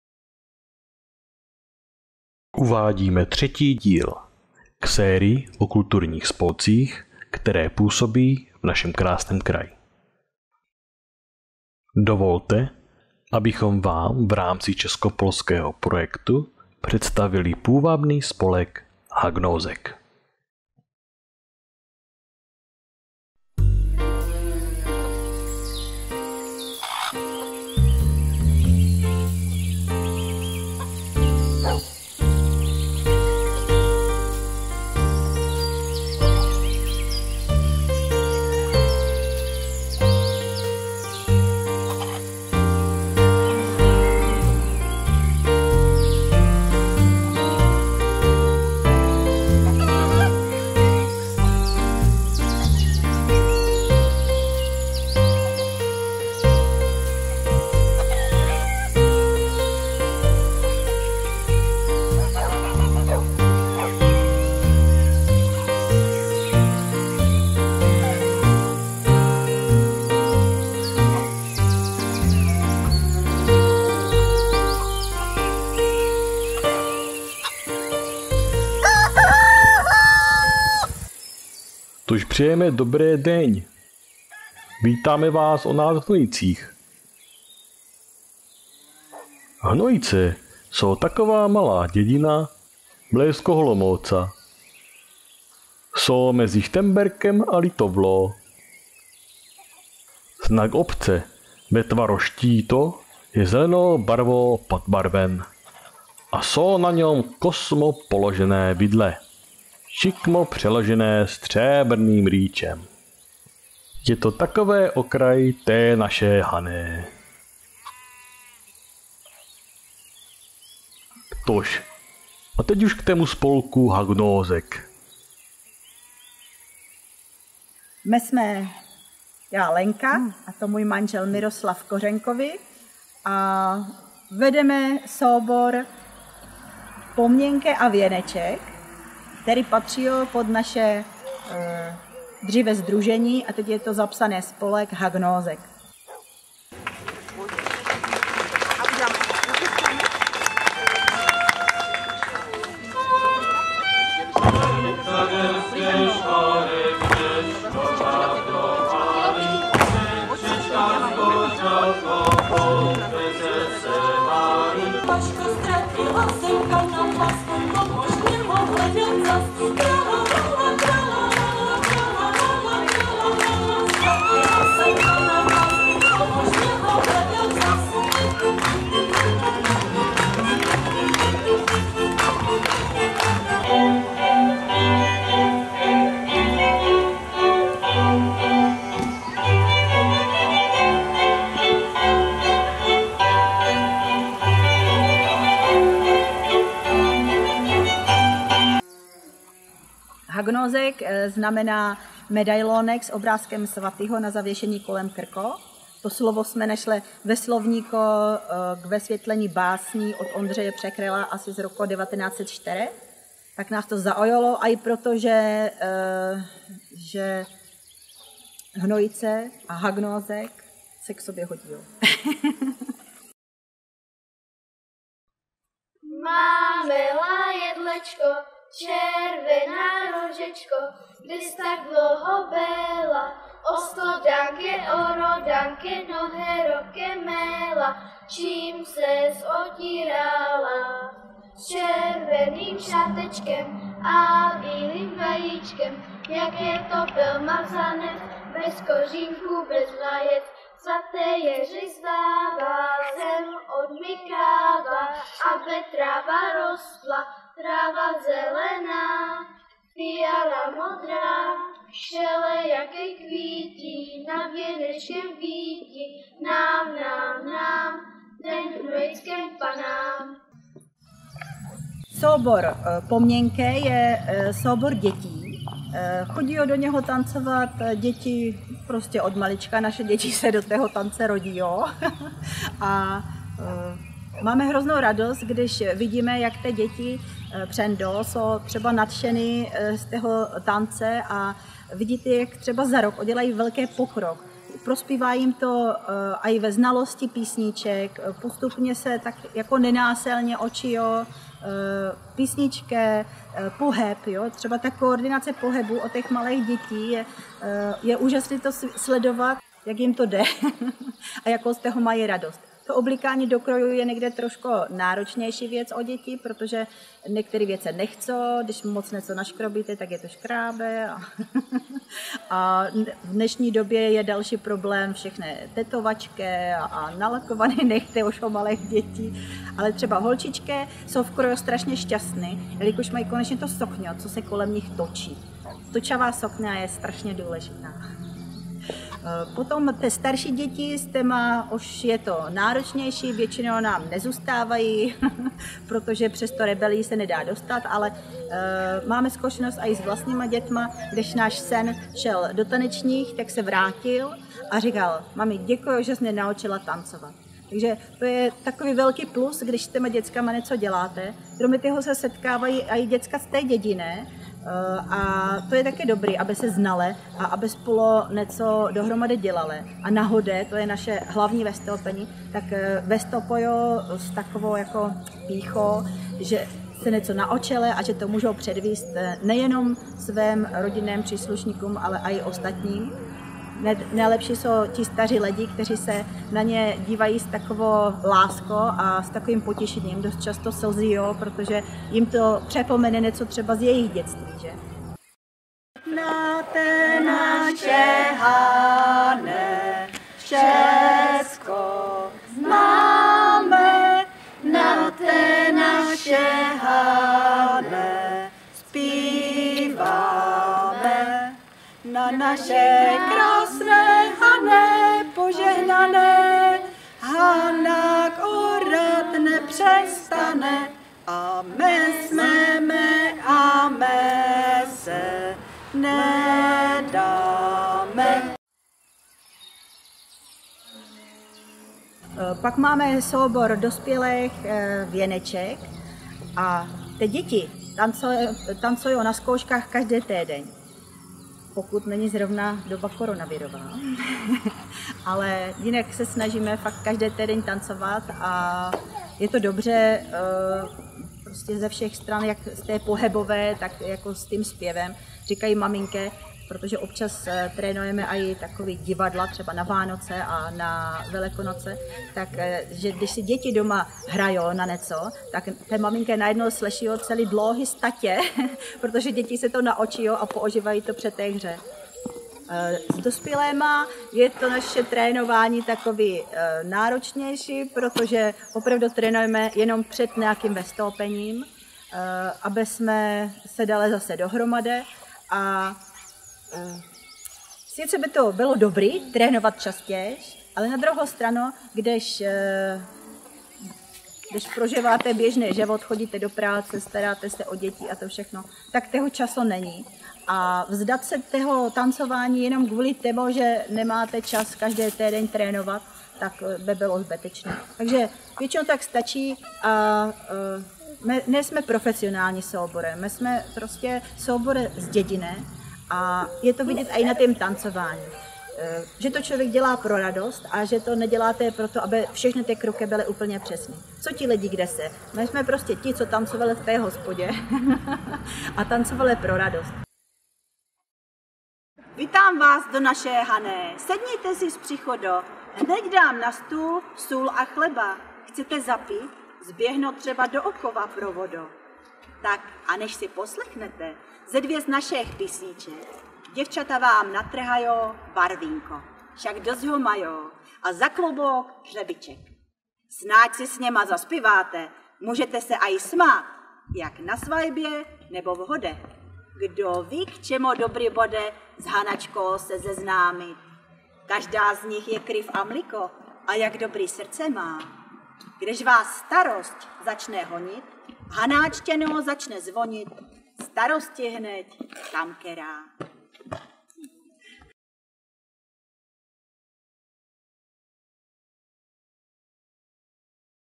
Folklorní spolek Hagnózek z Hnojic
Toto video jsme zpracovali pro MAS Šternbersko za účelem propagace města. Video je součástí projektu Česko-polské spolupráce. V tomto projektu jsme zpracovali celkem 5  videí výhradně o spolcích na území MAS Šternbersko.